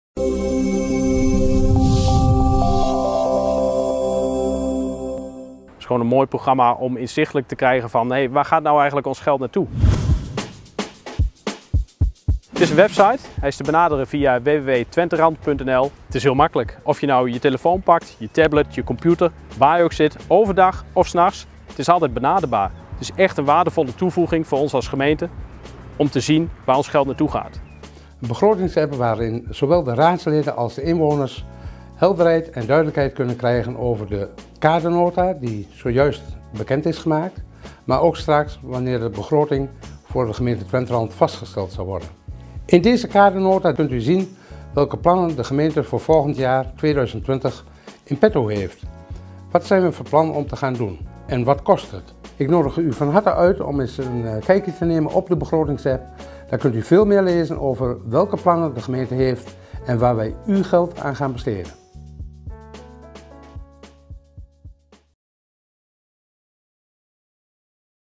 Twenterand: gemeente komt met BegrotingsApp
Via de begrotingsapp hebben raadsleden en inwoners inzicht in waar het geld van de gemeente aan uitgegeven wordt.
De app is te vinden via de website van de gemeente Twenterand